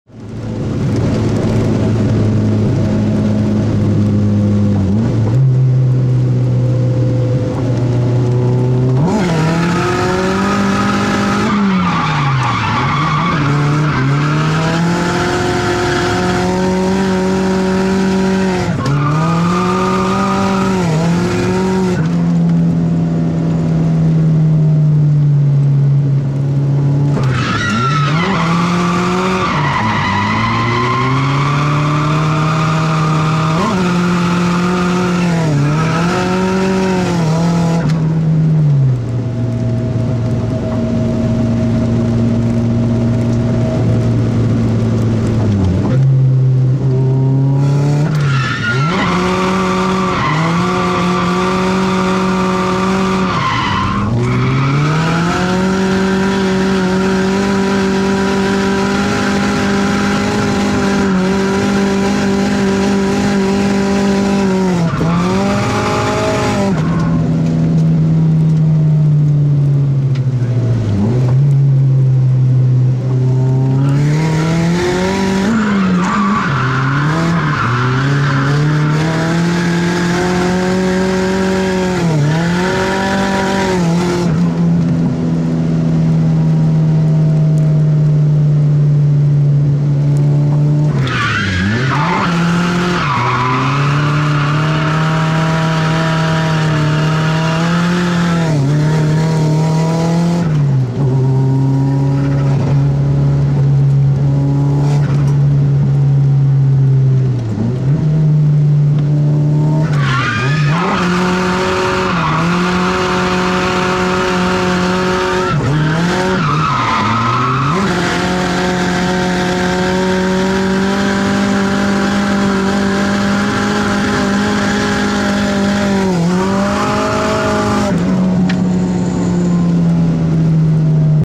Drifting with 180sx, DNSF Botniaring
Stock redtop SR20DET with gt2560 and external wg, 1,1bar boost.
ORC 409 clutch, welded diff.
Cusco coilovers (8kg/8kg springs) and Gktech RC correction kit